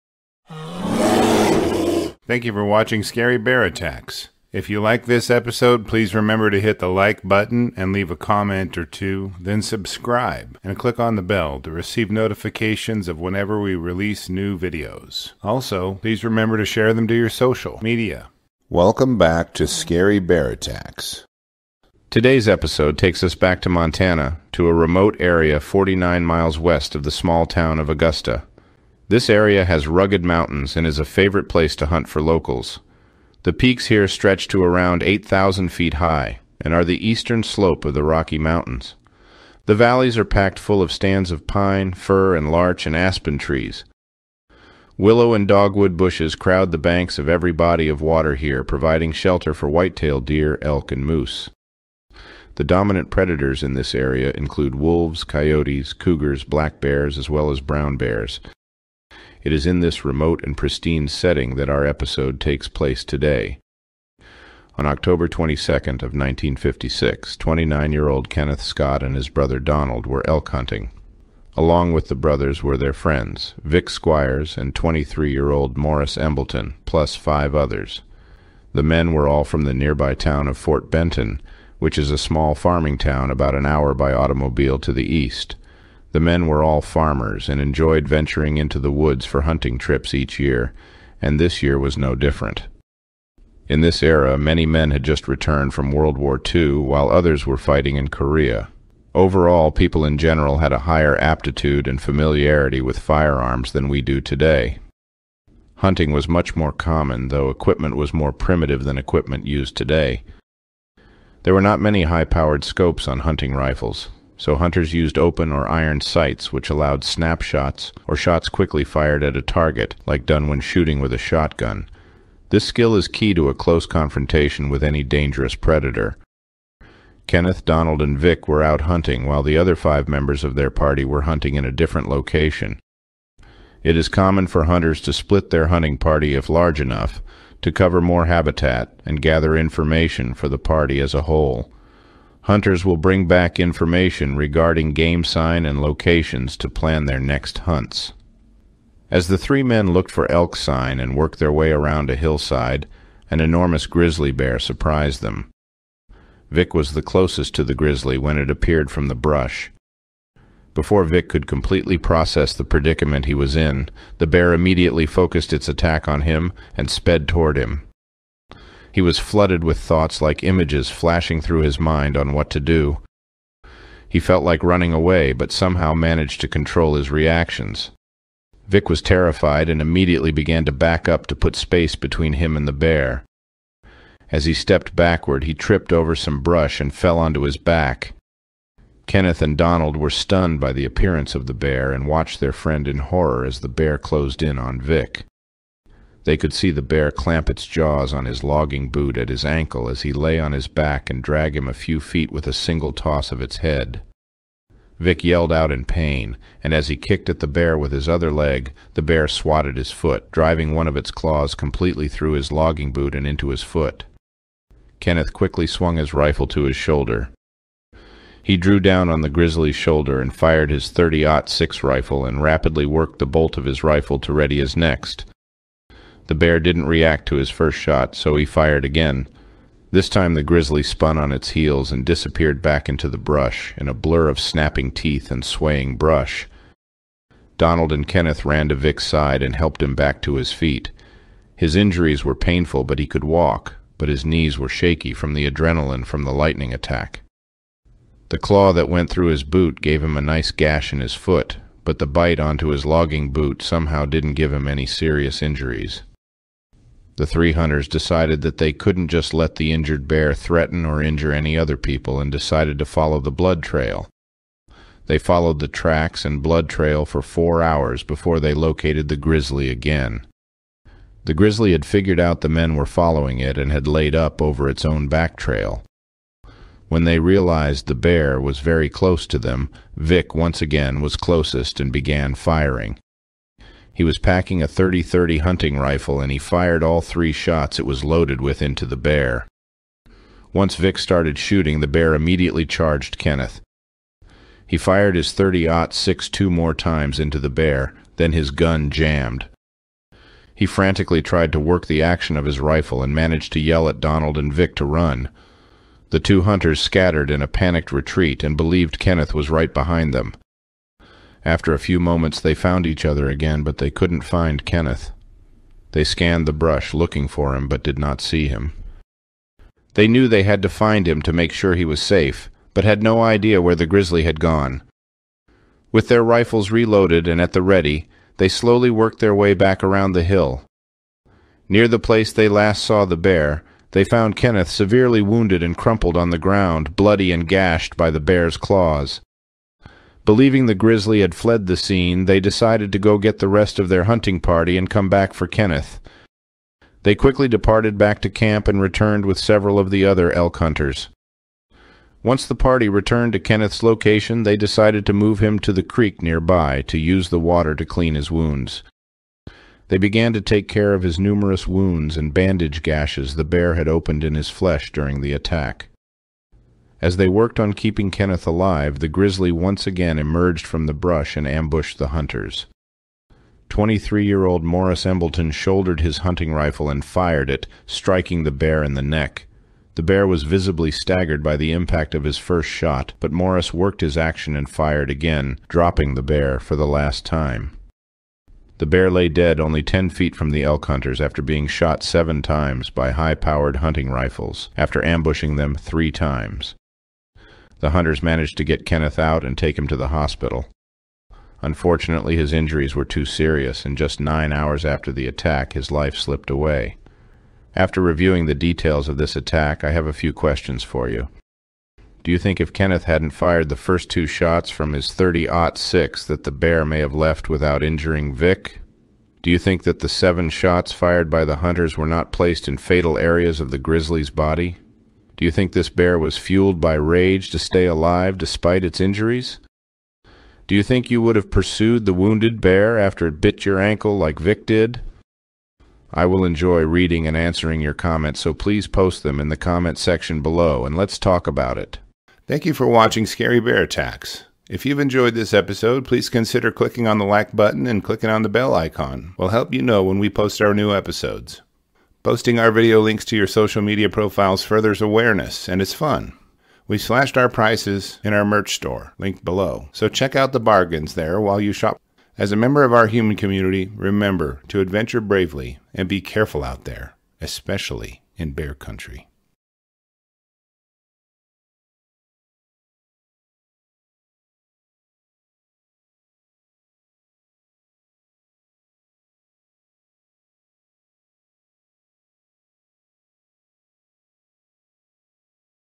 Grizzly Shot 9 Times With Rifle And Still Ambushes Elk Hunters 3 Times
Welcome back to Scary Bear Attacks! Today’s episode takes us back to Montana, to a remote area forty nine miles west of the small town of Augusta. This area has rugged mountains and is a favorite place to hunt for locals. The peaks here stretch to around eight thousand feet high and are the eastern slope of the Rocky Mountains. The valleys are packed full of stands of pine, fir and larch and aspen trees. Willow and dogwood bushes crowd the banks of every body of water here providing shelter for whitetail deer, elk and moose. The dominant predators in this area include wolves, coyotes, cougars, black bears as well as brown bears. It is in this remote and pristine setting that our episode takes place today.
On October twenty second, of nineteen fifty six, twenty nine year old Kenneth Scott and his brother Donald were elk hunting. Along with the brothers were their friends, Vic Squires and twenty three year old Morris Embleton, plus five others. The men were all from the nearby town of Fort Benton, which is a small farming town about an hour by automobile to the east. The men were all farmers and enjoyed venturing into the woods for hunting trips each year, and this year was no different.
In this era, many men had just returned from World War II while others were fighting in Korea. Overall, people in general had a higher aptitude and familiarity with firearms than we do today. Hunting was much more common though equipment was more primitive than equipment used today. There were not many high powered scopes on hunting rifles, so hunters used open or iron sights which allowed snap shots, or shots quickly fired at a target, like done when shooting with a shotgun. This skill is key to a close confrontation with any dangerous predator.
Kenneth, Donald and Vic were out hunting while the other five members of their party were hunting in a different location. It is common for hunters to split their hunting party if large enough, to cover more habitat and gather information for the party as a whole. Hunters will bring back information regarding game sign and locations to plan their next hunts. As the three men looked for elk sign and worked their way around a hillside, an enormous grizzly bear surprised them.
Vic was the closest to the grizzly when it appeared from the brush. Before Vic could completely process the predicament he was in, the bear immediately focused its attack on him and sped toward him. He was flooded with thoughts like images flashing through his mind on what to do. He felt like running away, but somehow managed to control his reactions. Vic was terrified and immediately began to back up to put space between him and the bear. As he stepped backward, he tripped over some brush and fell onto his back.
Kenneth and Donald were stunned by the appearance of the bear and watched their friend in horror as the bear closed in on Vic. They could see the bear clamp its jaws on his logging boot at his ankle as he lay on his back and drag him a few feet with a single toss of its head. Vic yelled out in pain and as he kicked at the bear with his other leg, the bear swatted his foot, driving one of its claws completely through his logging boot and into his foot.
Kenneth quickly swung his rifle to his shoulder. He drew down on the grizzlies shoulder and fired his thirty ott six rifle and rapidly worked the bolt of his rifle to ready his next. The bear didn’t react to his first shot, so he fired again. This time the grizzly spun on its heels and disappeared back into the brush, in a blur of snapping teeth and swaying brush.
Donald and Kenneth ran to Vic’s side and helped him back to his feet. His injuries were painful but he could walk, but his knees were shaky from the adrenalin from the lightning attack . The claw that went through his boot gave him a nice gash in his foot, but the bite onto his logging boot somehow didn’t give him any serious injuries.
The three hunters decided that they couldn’t just let the injured bear threaten or injure any other people and decided to follow the blood trail. They followed the tracks and blood trail for four hours before they located the grizzly again.
The grizzly had figured out the men were following it and had laid up over its own backtrail. When they realized the bear was very close to them, Vic, once again, was closest and began firing. He was packing a thirty thirty hunting rifle and he fired all three shots it was loaded with into the bear.
Once Vic started shooting, the bear immediately charged Kenneth. He fired his thirty ott six two more times into the bear, then his gun jammed. He frantically tried to work the action of his rifle and managed to yell at Donald and Vic to run.